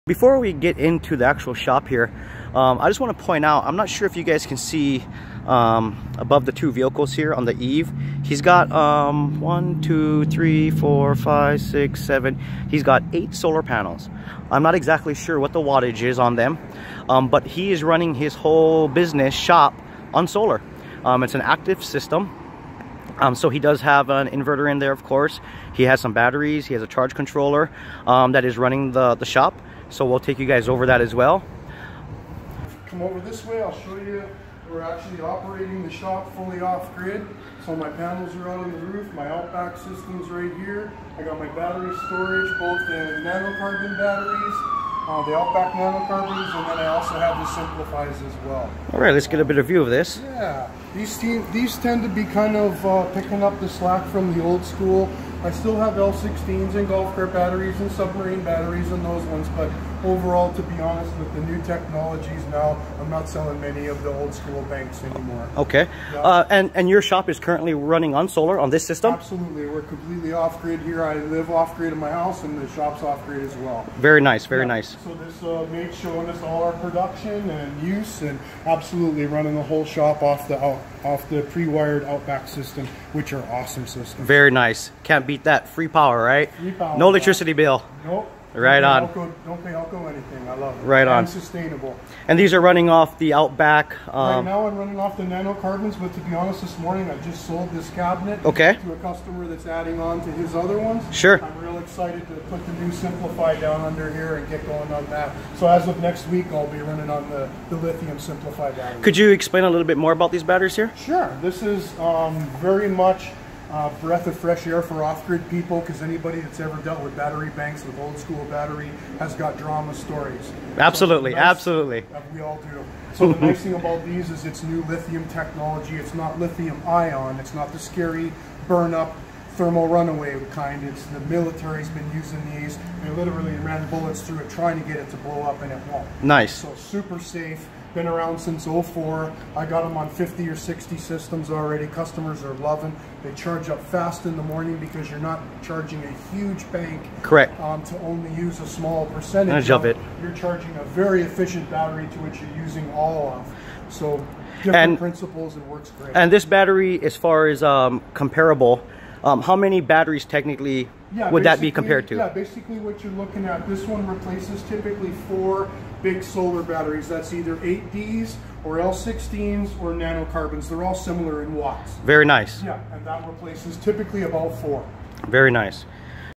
No Power Bill? No Problem. This Business Owner Runs His Shop 100% Off Grid!
"He's got 8 solar panels. He is running his whole business, shop on solar. It's an active system, so he does have an inverter in there of course. He has some batteries, a charge controller that is running the shop."

Makale: "We're actually operating the shop fully off grid, so my panels are out on the roof, my Outback system's right here. I've got  my battery storage - both in nanocarbon batteries, the Outback nanocarbons, and then I also have the SimpliPhi as well. These tend to be picking up the slack from the old school. I still have L16s & golf cart batteries & submarine batteries and those ones, but overall, to be honest, with the new technologies now, I'm not selling  many of the old school banks anymore. We're completely off grid here. I live off grid at my house and the shop's off grid as well. So this Mate's showing us all our production & use. Absolutely, running the whole shop off the pre-wired Outback system, which are awesome systems. Very nice. Can't be that. Free power, right? No electricity bill."